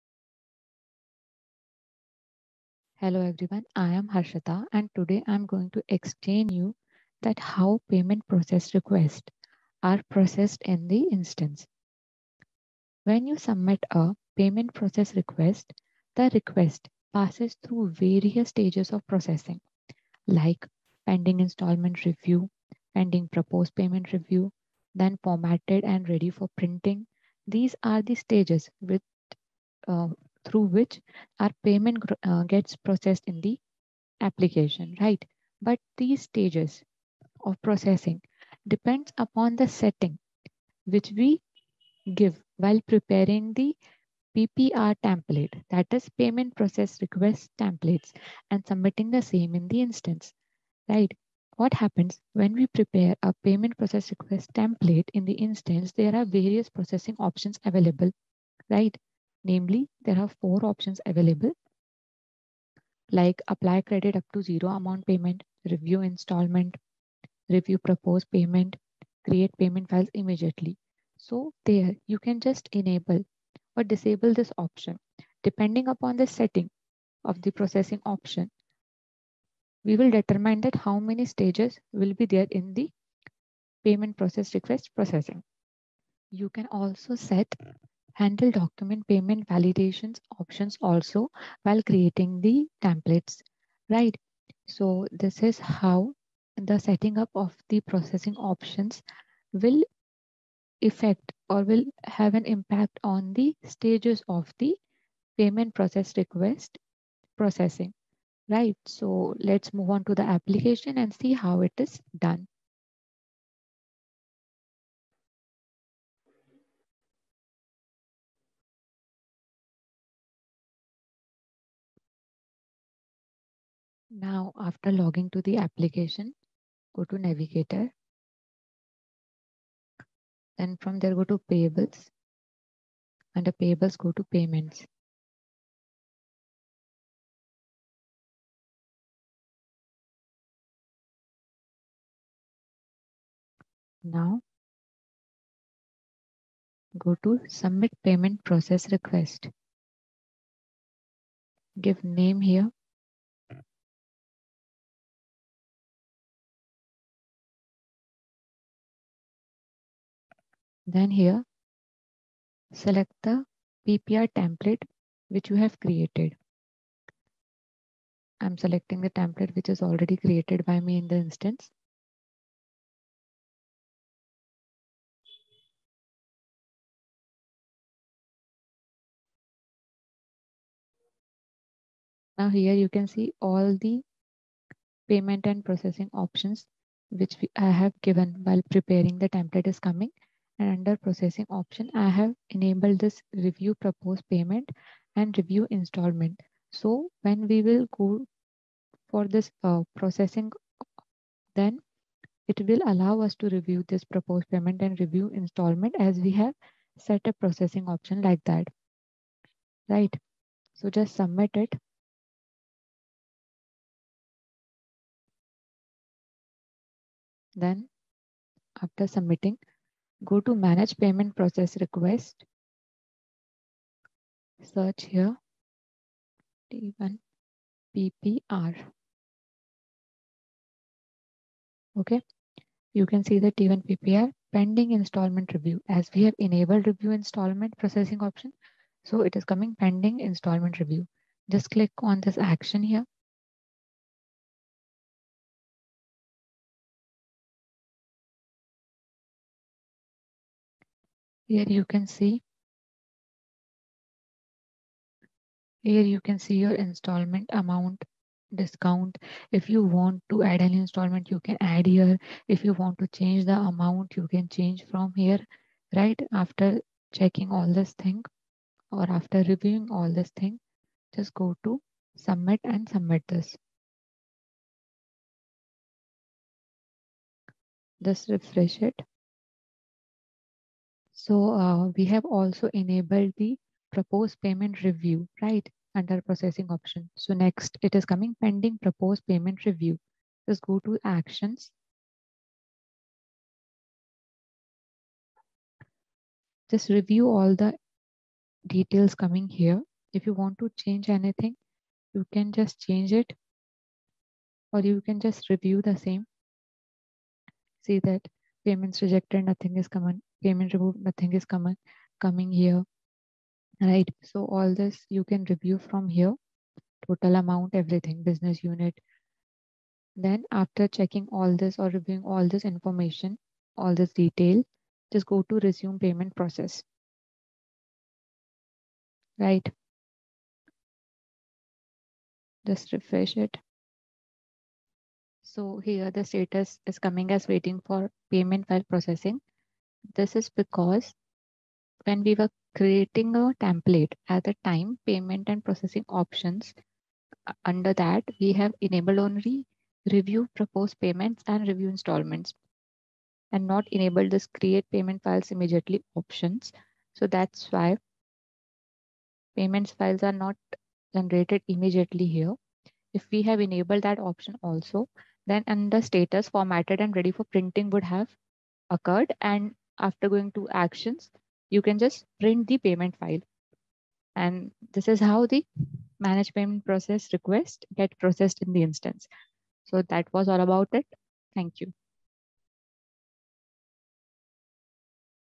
How Payment Process Requests are Processed in Oracle Fusion AP?|BISP
A payment process request is a group of installments submitted for payment. You can submit a payment process request on the Submit Payment Process Request page. You can also create a template on the Create Payment Process Request Template page and schedule the template to submit the payment process request later.
In this video tutorial, we will explain step by step How Payment Process Requests are Processed in Oracle Fusion AP?
0:00 How Payment process request are processed
2:01 How Payment process request is processed explain with example

list=PL7CWBDRZZ_Qd0FQQ5J-npMFcjzQTgs5dl